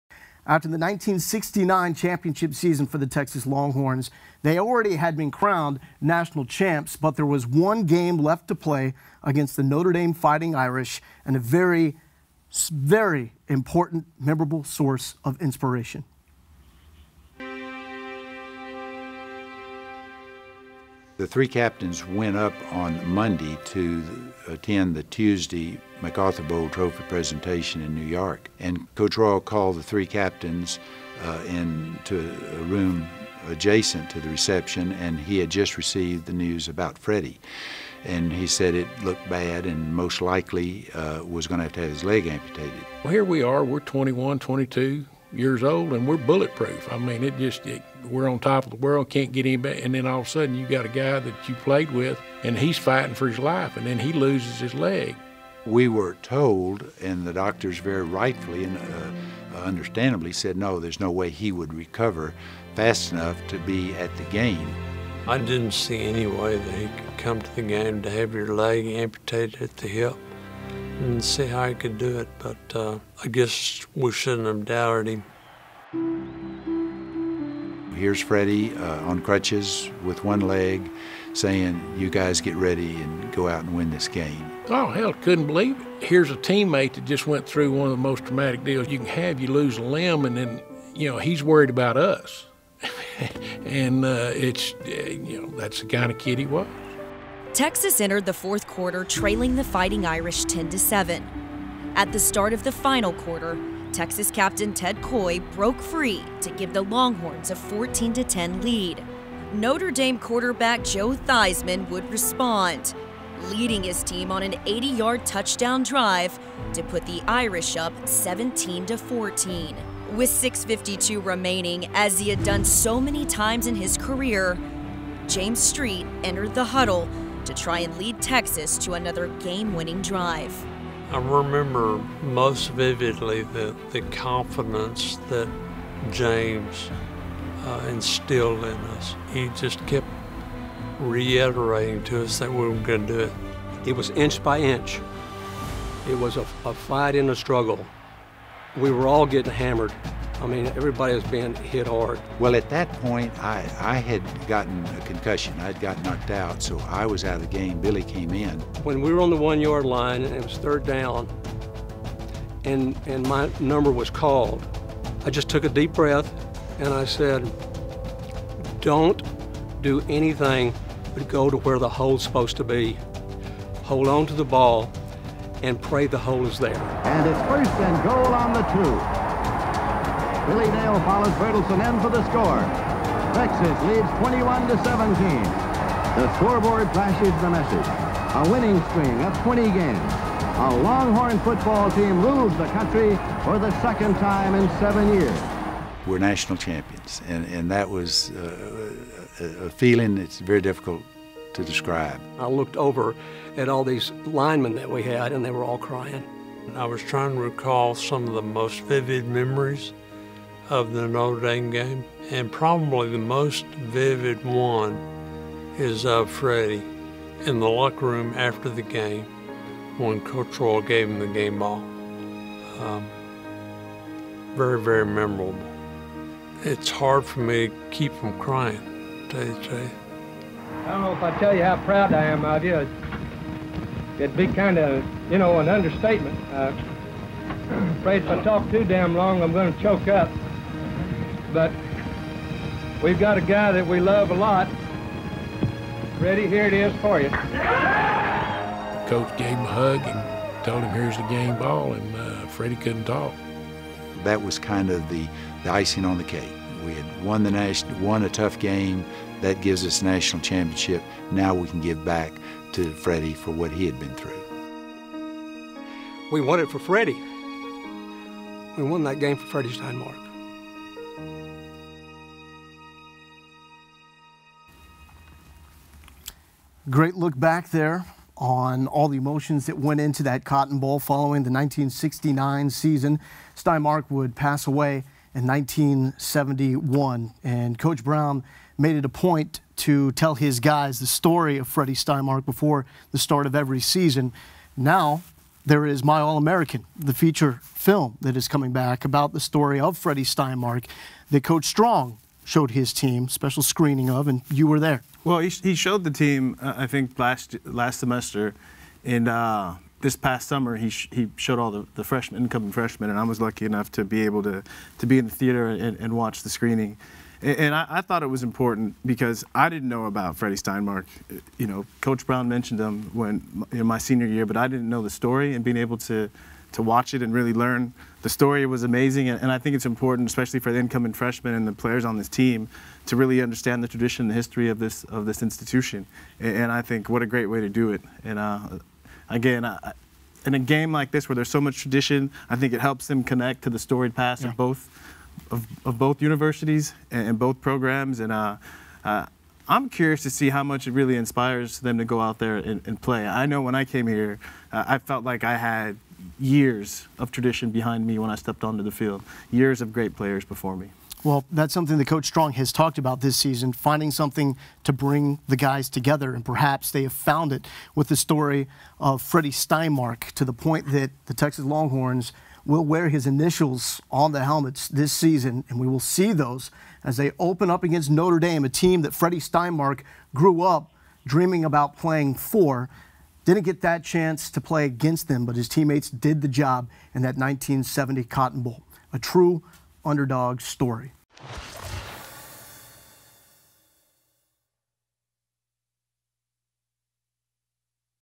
Freddie Steinmark tribute and impact [Sept. 19, 2015]
Watch Longhorn Network's Freddie Steinmark tribute featuring Longhorn legends discussing his impact on them and UT.